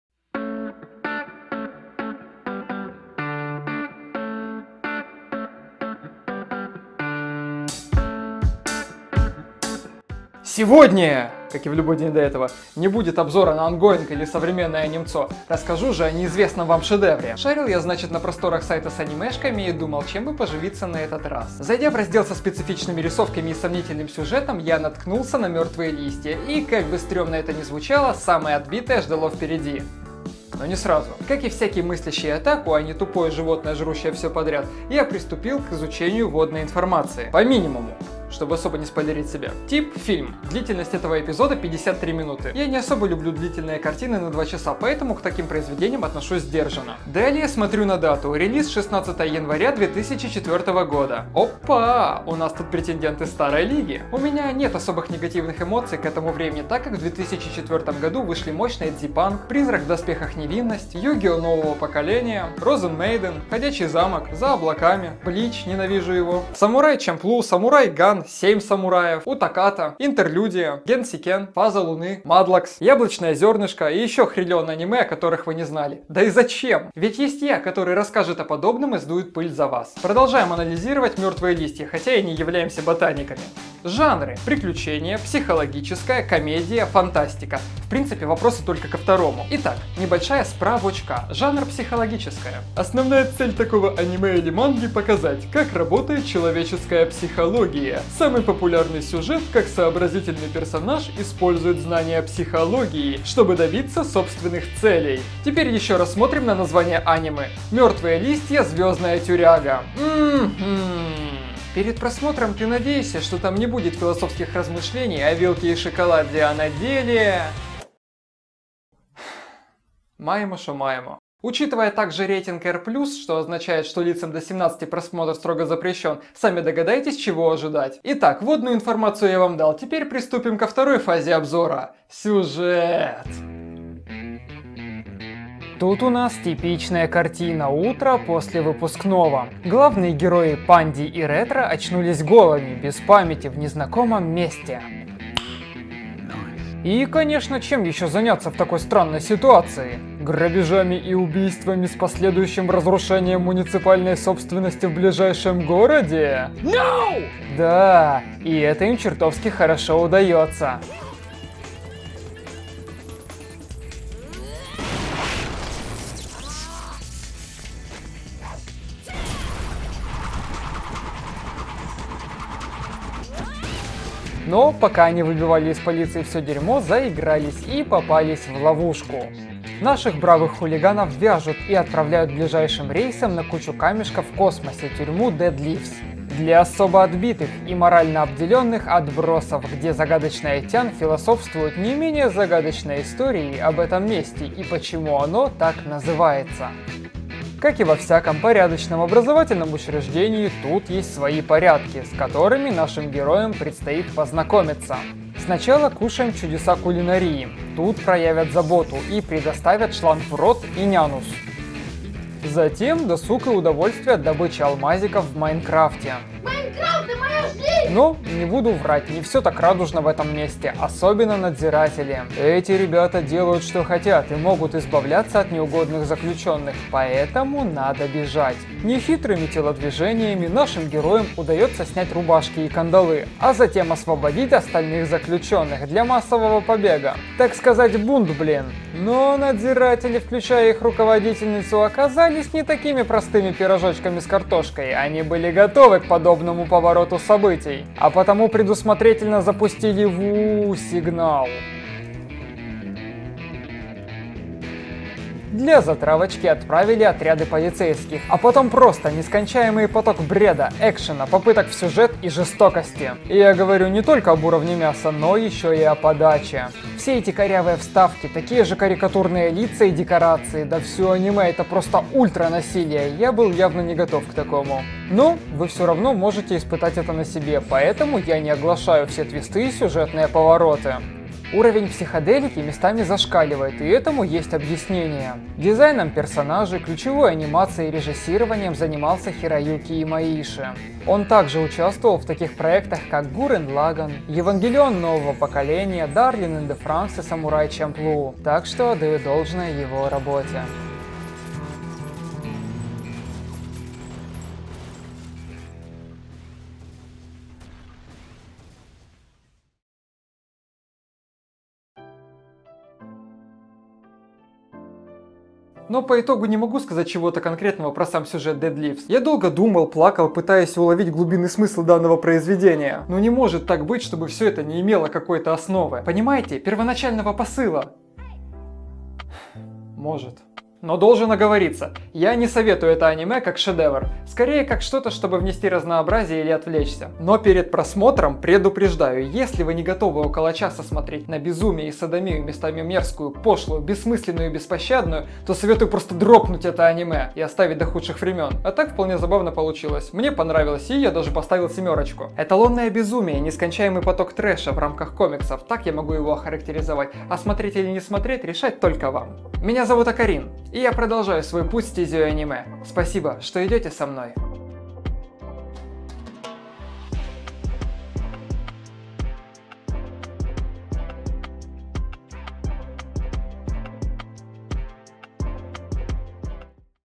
ОБЗОР АНИМЕ DEAD LEAVES ☆ МЕРТВЫЕ ЛИСТЬЯ: ЗВЕЗДНАЯ ТЮРЯГА
МЕРТВЫЕ ЛИСТЬЯ - это действительно артхаус в мире аниме, который хоть и редко, но встречается в ТОПах. История цепляет своей странностью с самого начала: главные герои Панди и Ретро очнулись нагишом в незнакомом месте, а также без памяти. Но, вместо выяснения кто они такие, им пришла в голову мысль начать разбой и грабежи в ближайшем городе, за что их и упекли в тюрьму DEAD LEAVES для особо опасных преступников. А как им предстоит оттуда сбежать - смотрите фильм 2004 года МЕРТВЫЕ ЛИСТЬЯ: ЗВЕЗДНАЯ ТЮРЯГА.
Специфичная рисовка, жестокость, нескончаемый поток юмора, треша и угара!

                                                  .•°*”˜˜”*°•♥●•٠٠•●♥°*”˜˜”*°•
Поддержать Акарина или заказать обзор:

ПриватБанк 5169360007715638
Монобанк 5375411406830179